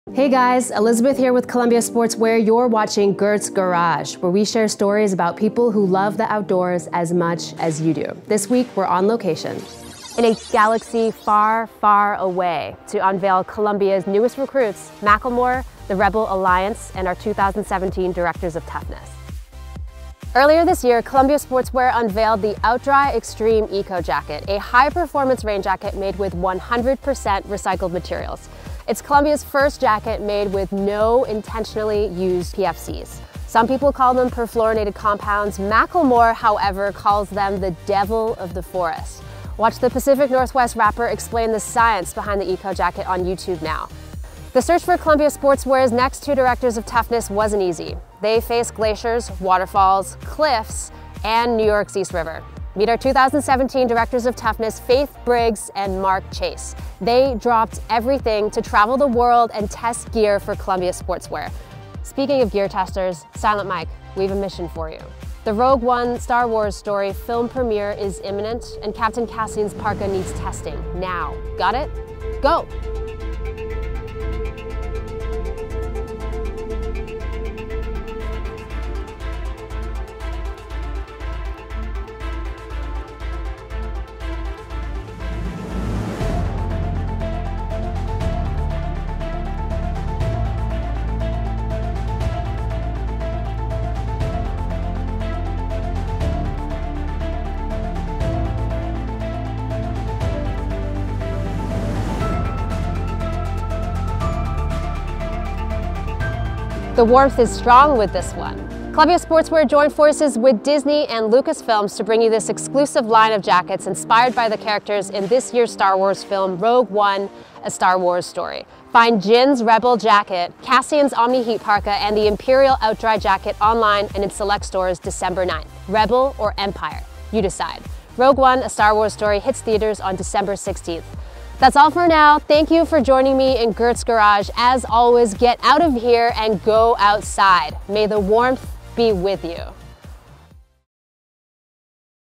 Tested Tough by Rebels in a Galaxy Far, Far Away....
This week on Gert's Garage we introduce Columbia's newest recruits: Macklemore, the Directors of Toughness and the Rebel Alliance.